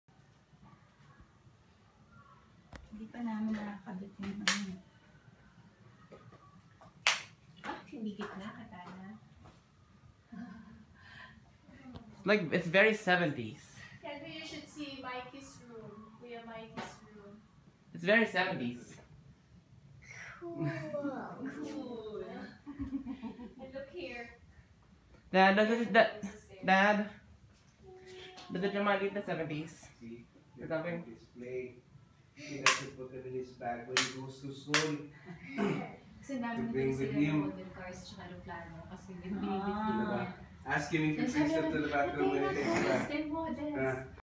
Retro-style airplane chandelier/light fixture in a bedroom in our friends' house
This video was recorded during our Christmas vacation to the Philippines.

This is the son's bedroom